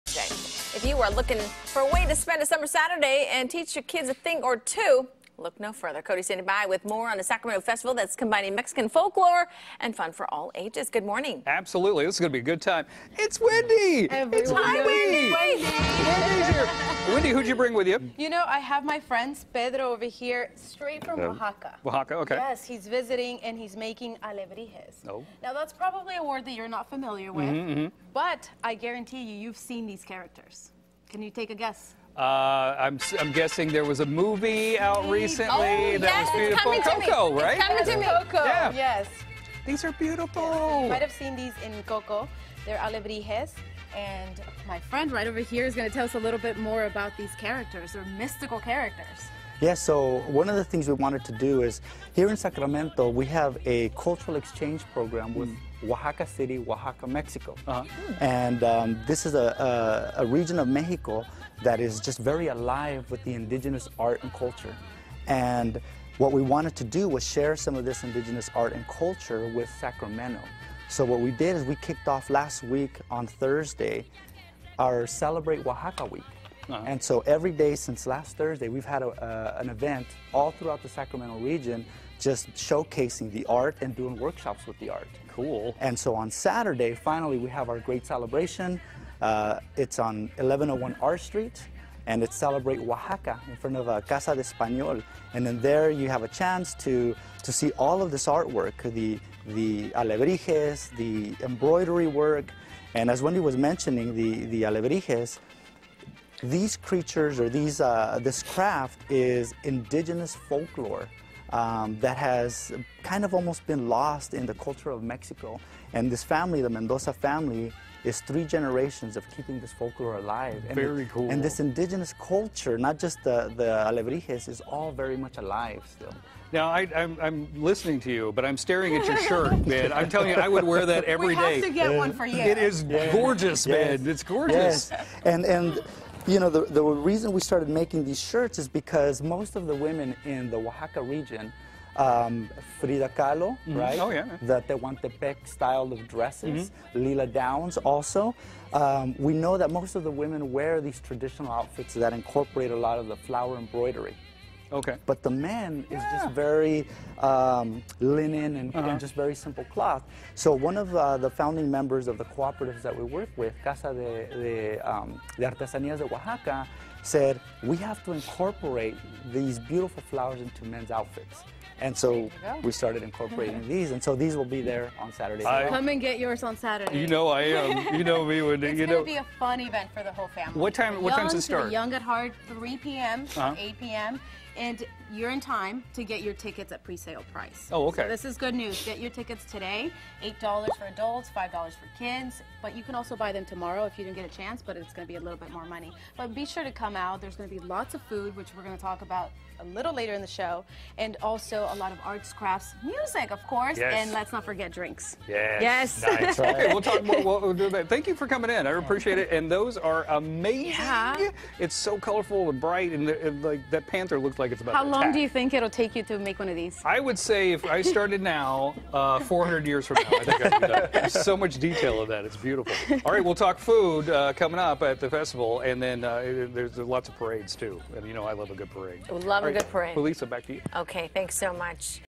Oaxaca Festival
Cody is learning about the Oaxaca Festiva that will celebrates a region in Mexico bringing a series of free events all leading up to the Celebrate OaxaCA!